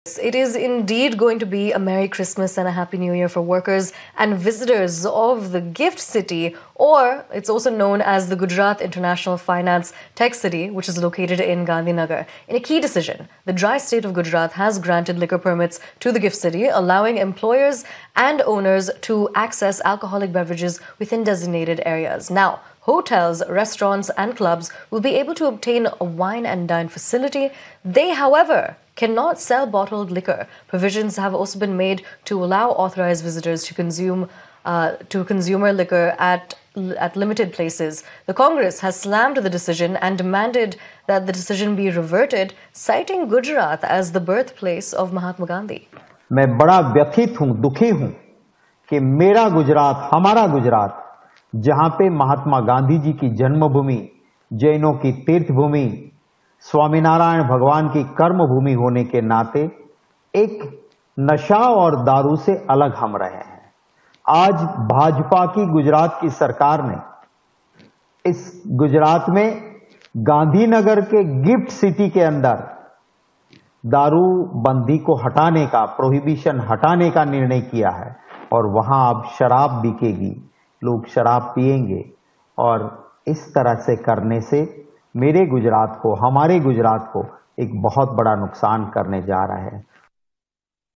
Gujarat govt grants permission for consumption of liquor in GIFT City
In a notable development, individuals employed in dry state Gujarat's GIFT City (Gujarat International Finance Tec-City) and authorized visitors will now be granted liquor permits, allowing them access to alcoholic beverages within the designated area. However, while hotels within GIFT City are permitted to serve liquor, they are restricted from selling bottled alcohol as per the latest regulations.